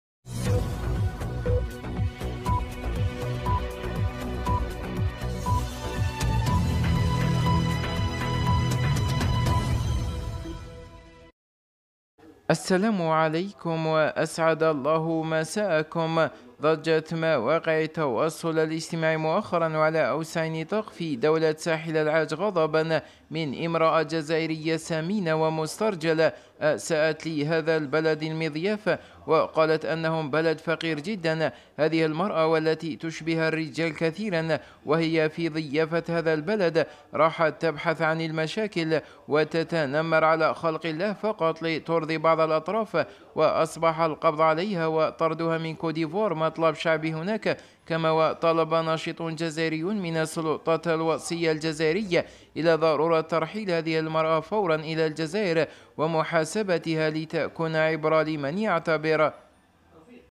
عاجل 🔴 القبض على الجزائرية صوفيا بن لمان في كوديفوار اصبح مطلب شعبي...لقاوها تعاير فيهم بالفقراء
فيديو تصريحات صوفيا بن لمان في ساحل العاج كوديفوار